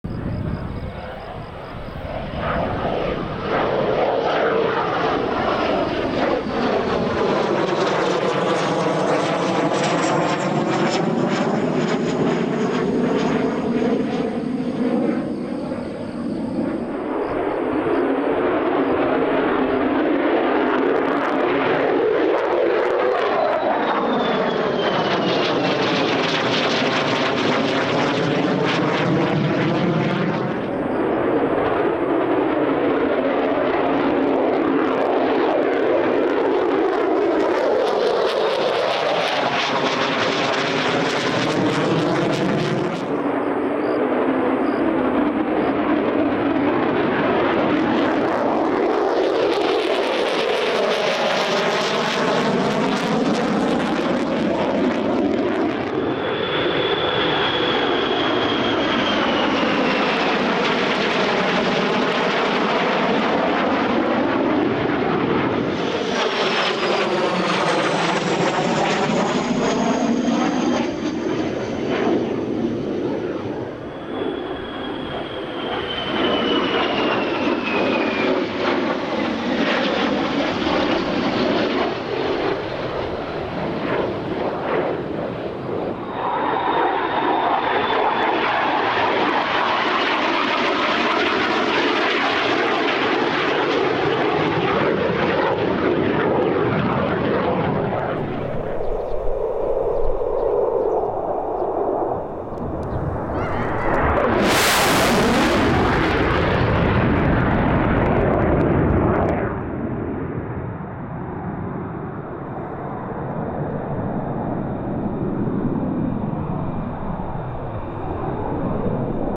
2022.9.5 三沢基地 F-35A T-4 3年ぶりの航空祭へ向けオープニングフライト訓練！！
少しでも楽しんでいただけたら幸いです。